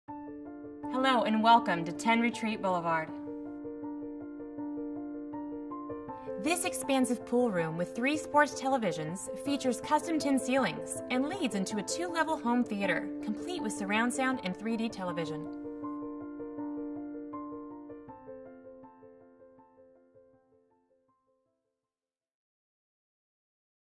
Billiards Room & Theatre, 10 Retreat, Luxury Houston Estate
Beautiful interior video of 10 Retreat Boulevard, Richmond, Texas. 10retreat.com